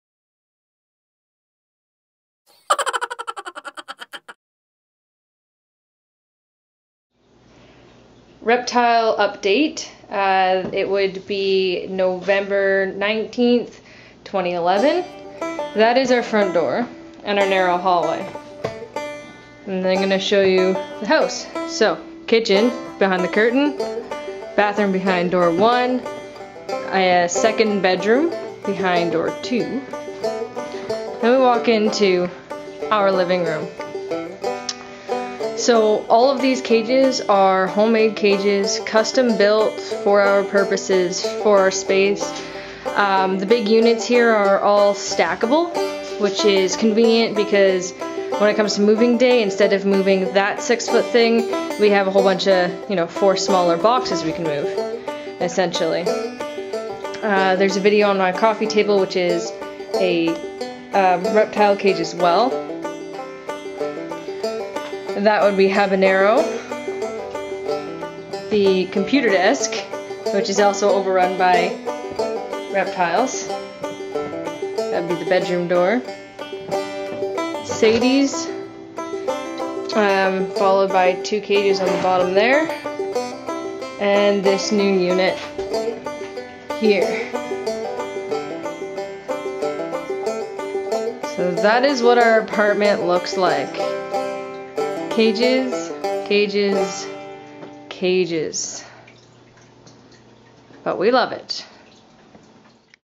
Reptile Room- Showing off the New Cages
Have a look at what our place looks like with all of our pets.